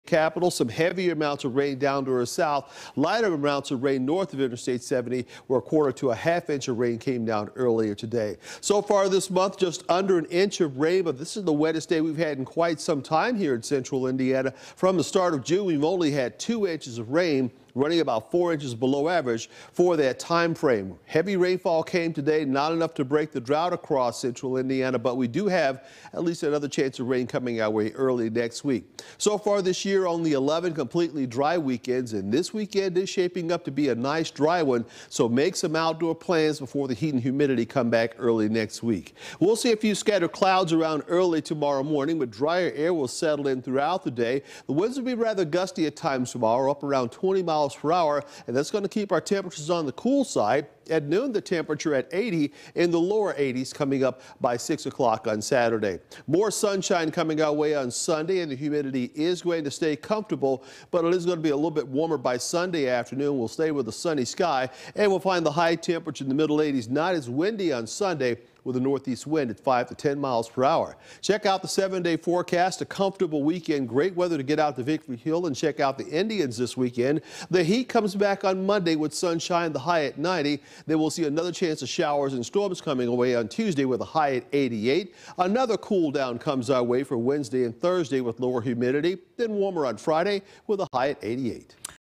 Chris forecasts a sunny weekend
Expect sunny skies and lower humidity this weekend.